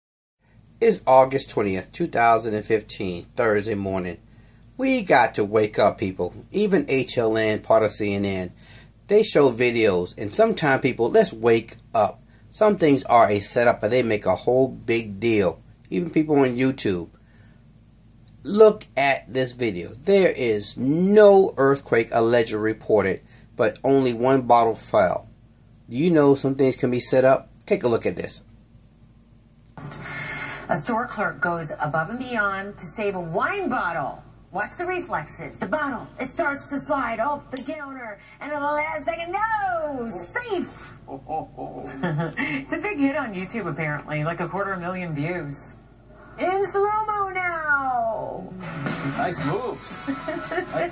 Wine Bottle Catch? SET IT!!
20 August 2015 HLN News. People..please wake the Duck up. An host on cable..she makes this video sound so Xciting how someone can do this. (Please..everyone. .take a Close look at it). No other bottle is shaking. Not even..the store. So..this Clerk..catch a bottle of wine..that is Lrss than $15 dollars.  WHY? * Can ya say..SET UP..for View 2 Viral ultimatum*